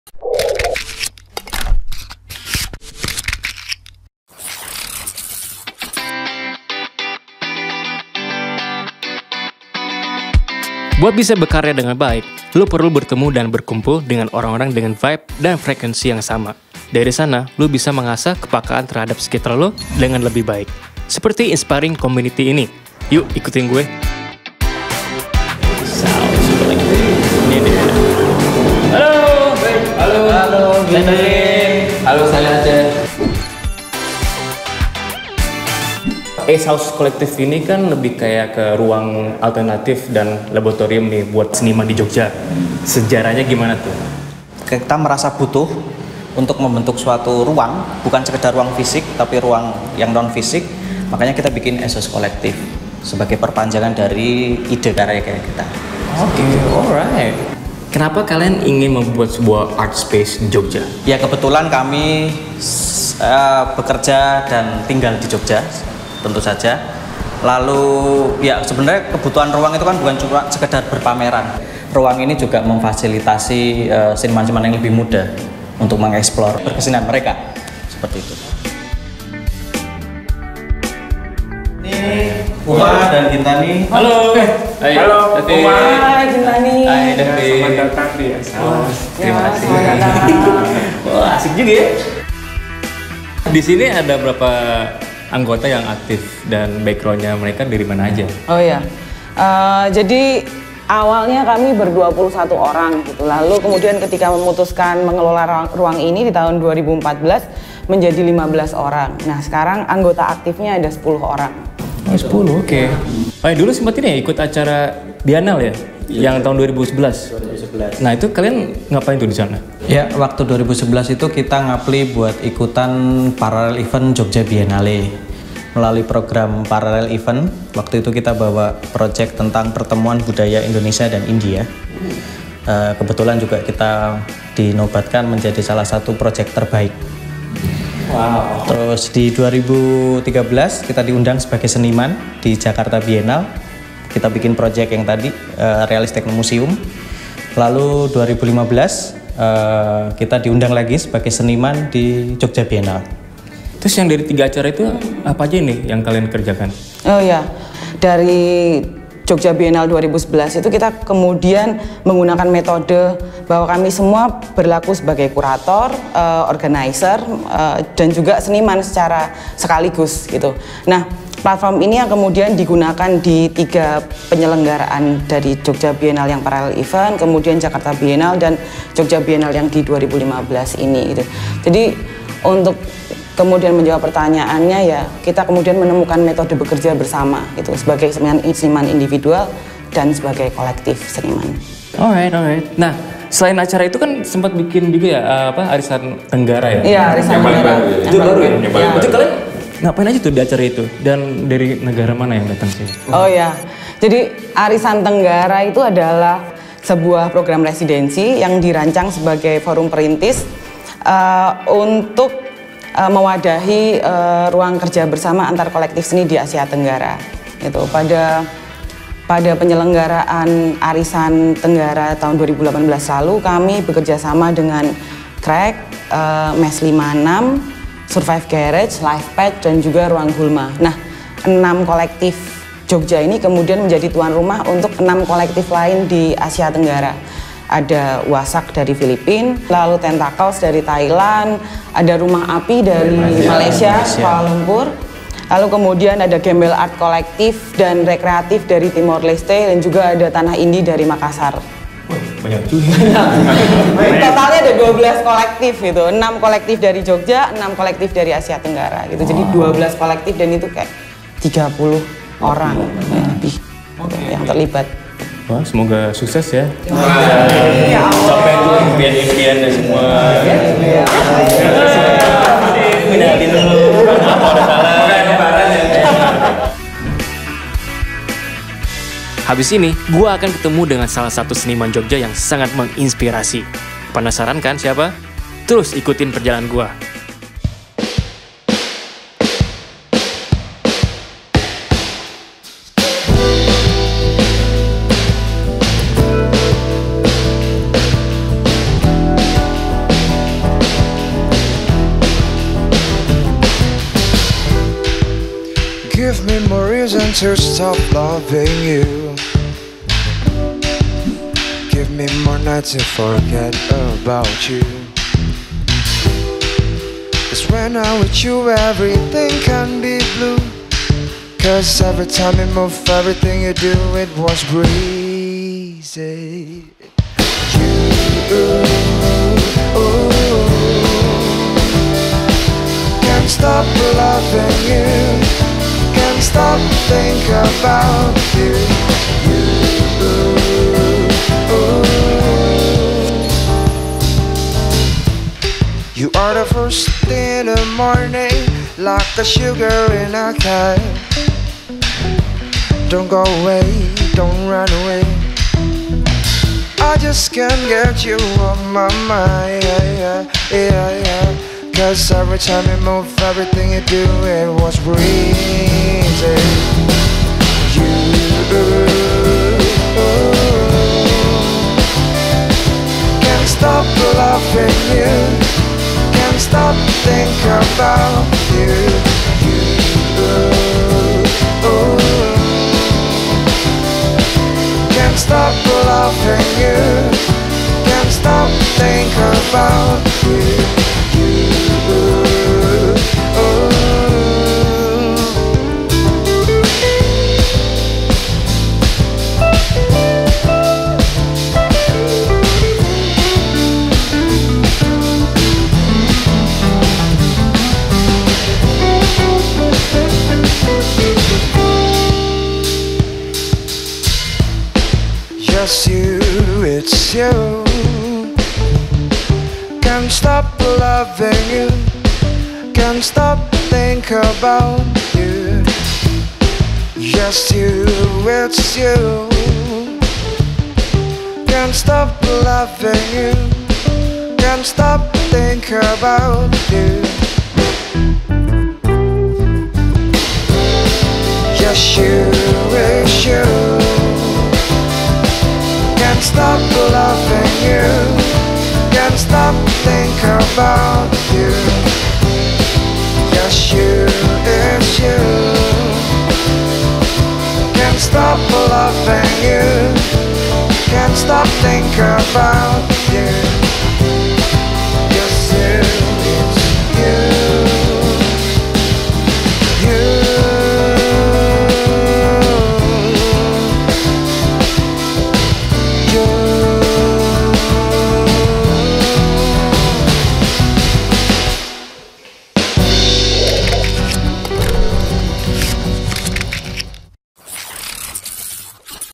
KISAH DI SUDUT JOGJA: Ruang Buatan Ace House Collective | Inspiring Communities #13
Berawal dari kebutuhan ruang untuk eksplorasi seni, tercipta lah Ace House Collective. Project mereka telah menyambangi berbagai Biennale di Jogja dan Jakarta. Mereka pun menggelar sebuah kelas seni dengan skala besar, se-Asia Tenggara! Seperti apa idealisme dan perspektif mereka dalam berseni? Simak video ini!